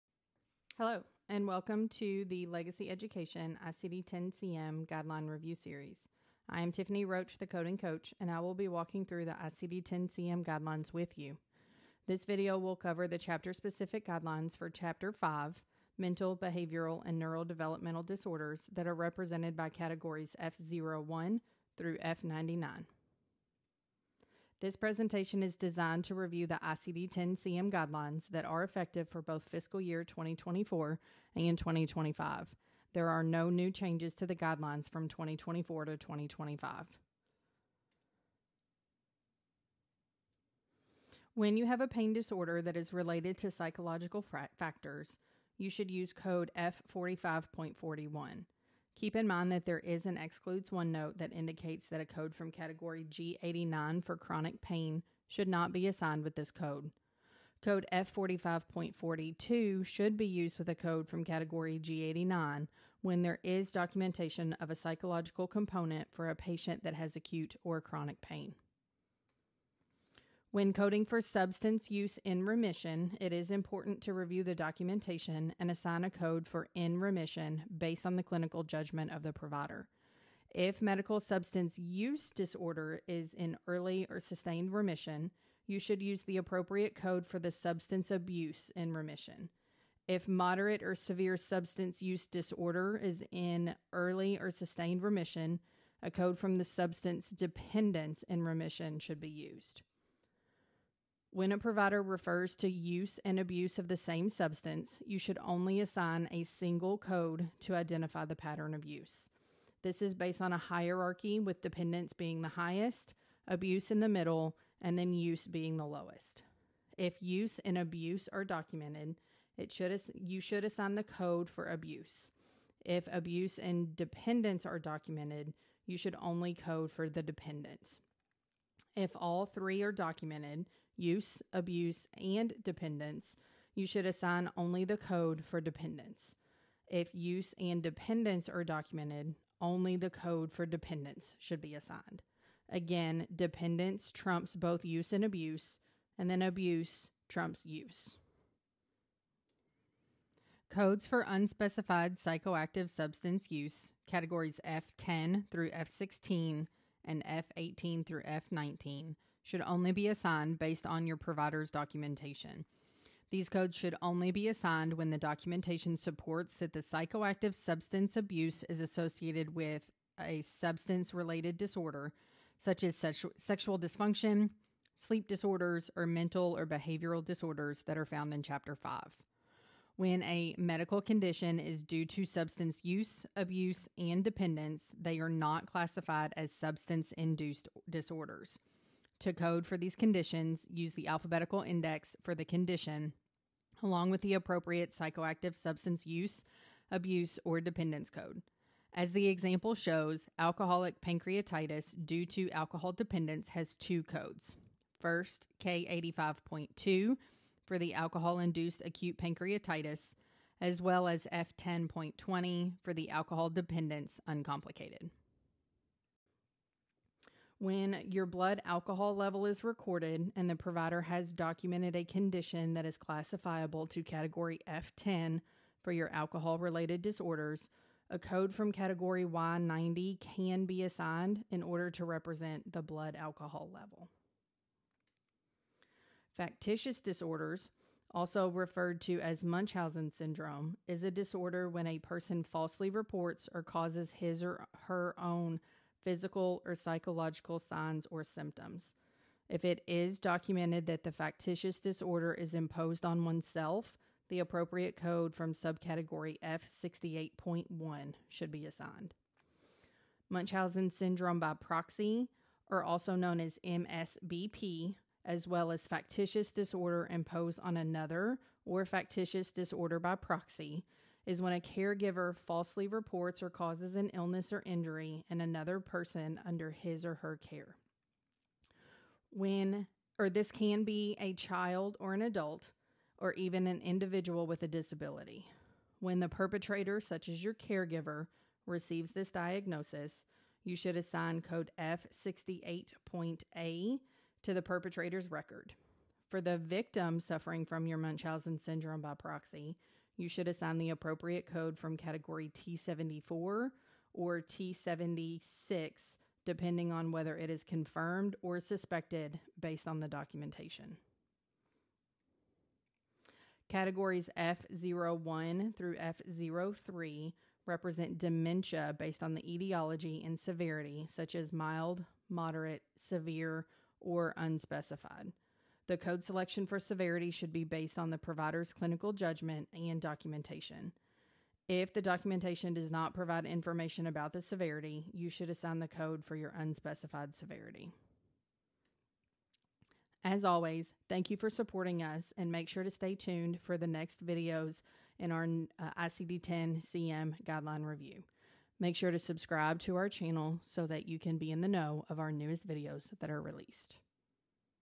ICD-10-CM Guideline Review: Chapter 5 Mental, Behavioral & Neurodevelopmental Disorders
Join us for a breakdown of the Chapter 5 Mental, Behavioral & Neurodevelopmental Disorders

This is a part of our ICD-10-CM Guideline Review Series.  Guidelines can be found within your ICD-10-CM manual or on the CMS website.